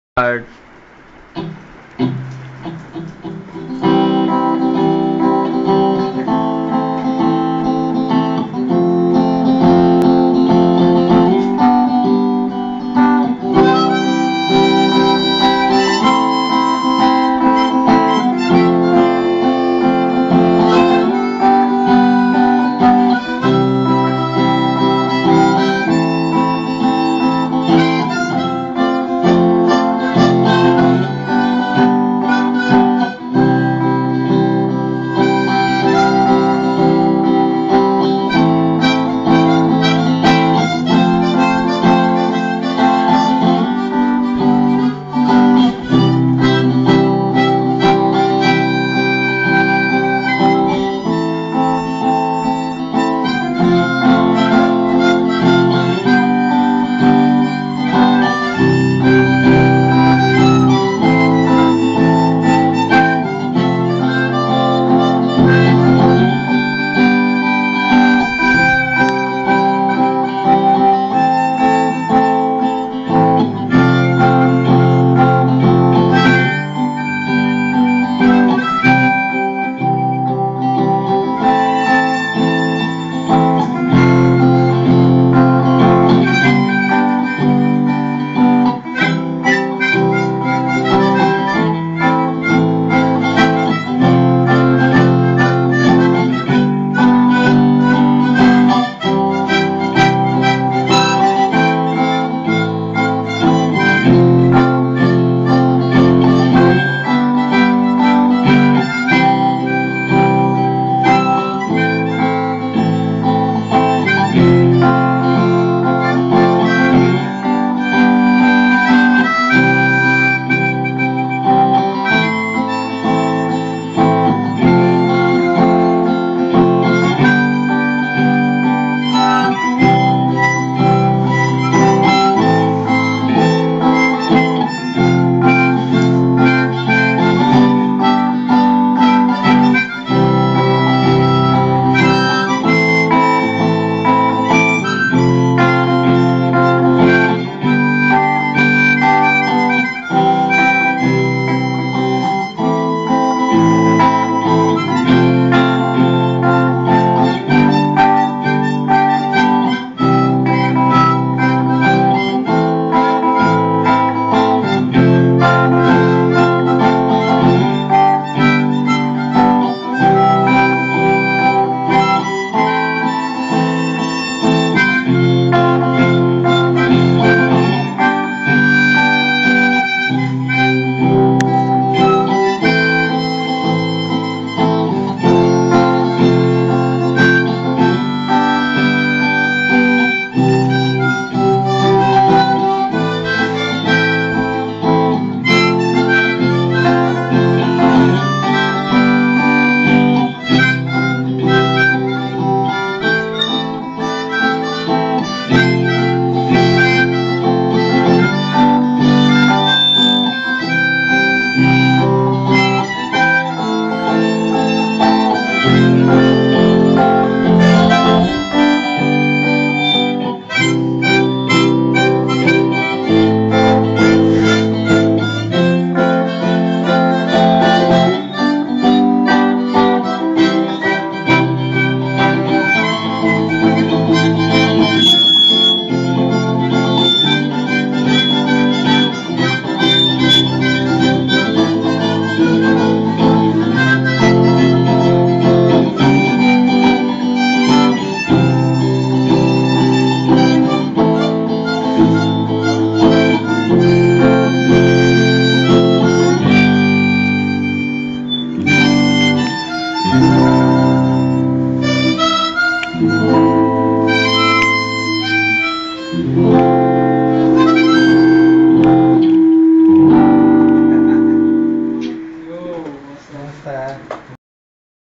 Mouth Organ at IIT Roorkee Ganga Bhawan
OC by Null & Void at IIT R Mouth Organ: Gaurav Guitar: Shivam (Champs)